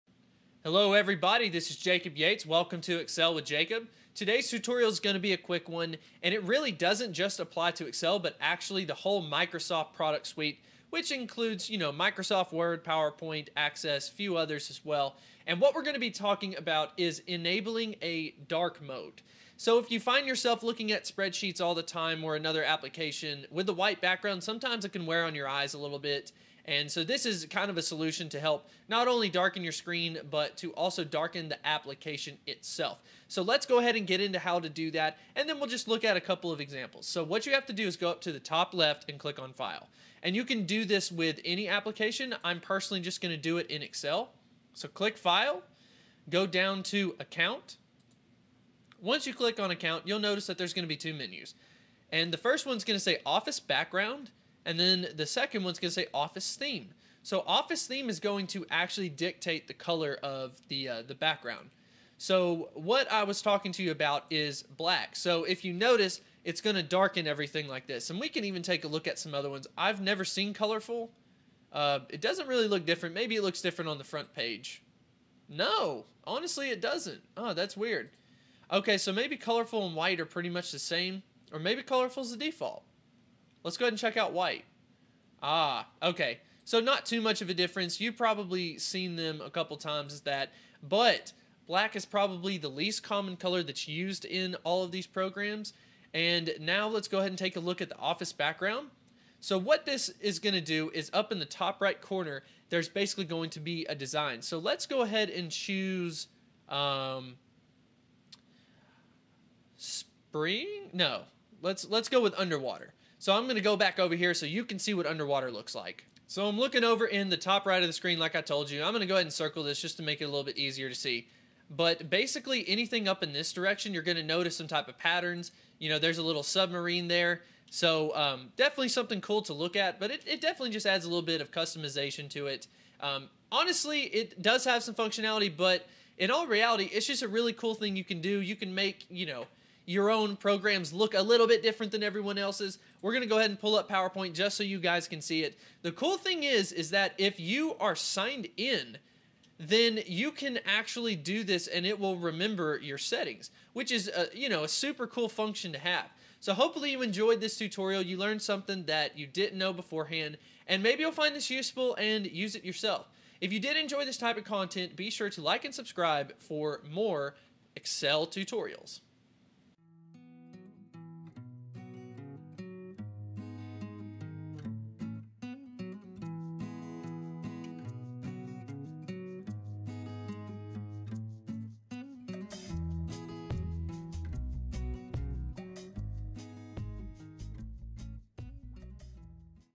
How To Change The Theme On Microsoft Office Products 2016 Dark Theme
Hello Everyone and welcome to Excel with Jacob. In this episode I talk about how to change the theme on your Microsoft Office products to a "dark mode". Many are not aware that they can change the color of the background to black, and with the new update of signing in, you can have it saved as your default settings.
meaning that the settings can follow you wherever you go.

Definitely a great feature for people whose eyes hurt from staring at a white screen for so long, plus it looks cool :)

Excel , PowerPoint, Access, Microsoft Word, and more 2016.